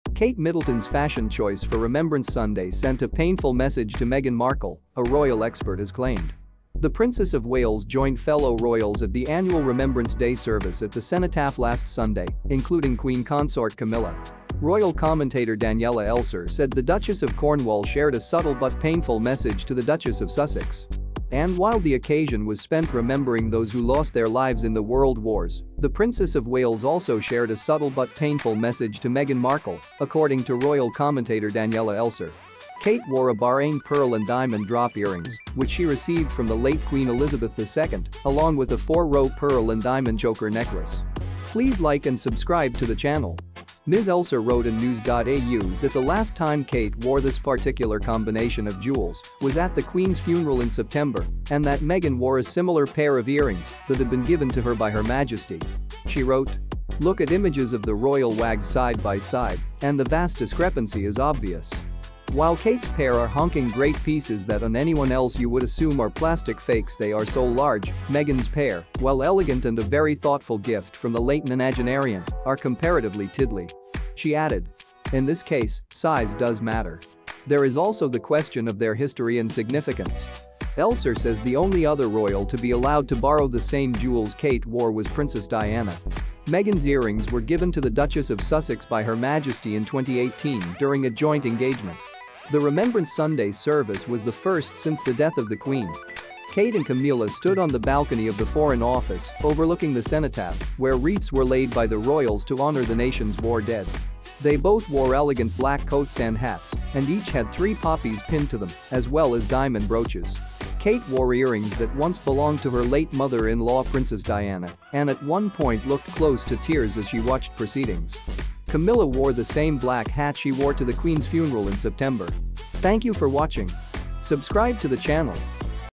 ROYAL FAMILY NEWS! Catherine, Princess of Wales sends Meghan Markle a 'painful' message
Catherine, Princess of Wales sends Meghan Markle a 'painful' message

Kate Middleton's fashion choice for Remembrance Sunday sent a "painful" message to Meghan Markle, a royal expert has claimed.
The Princess of Wales joined fellow royals at the annual Remembrance Day Service at The Cenotaph last Sunday including Queen Consort Camilla.
Royal commentator Daniela Elser said the Duchess of Cornwall shared a subtle but "painful" message to the Duchess of Sussex.
And while the occasion was spent remembering those who lost their lives in the World Wars, the Princess of Wales also shared a subtle but "painful" message to Meghan Markle, according to royal commentator Daniela Elser.
Kate wore a Bahrain pearl and diamond drop earrings, which she received from the late Queen Elizabeth II, along with a four-row pearl and diamond choker necklace.
Ms Elser wrote in news.au that the last time Kate wore this particular combination of jewels was at the Queen's funeral in September, and that Meghan wore a similar pair of earrings that had been given to her by Her Majesty.
She wrote: "Look at images of the royal WAGs side-by-side and the vast discrepancy is obvious.
"While Kate’s pair are honking great pieces that on anyone else you would assume are plastic fakes they are so large, Meghan’s pair, while elegant and a very thoughtful gift from the late nonagenarian, are comparatively tiddly."
She added: "In this case, size does matter. There is also the question of their history and significance."
Elser says the only other royal to be allowed to borrow the same jewels Kate wore was Princess Diana.
Meghan's earrings were given to the Duchess of Sussex by Her Majesty in 2018 during a joint engagement.
The Remembrance Sunday service was the first since the death of the Queen.
Kate and Camila stood on the balcony of the Foreign Office, overlooking the Cenotaph, where wreaths were laid by the royals to honour the nation's war dead.
They both wore elegant black coats and hats and each had three poppies pinned to them as well as diamond brooches.
Kate wore earrings that once belonged to her late mother-in-law Princess Diana and at one point looked close to tears as she watched proceedings. Camilla wore the same black hat she wore to the Queen's funeral in September.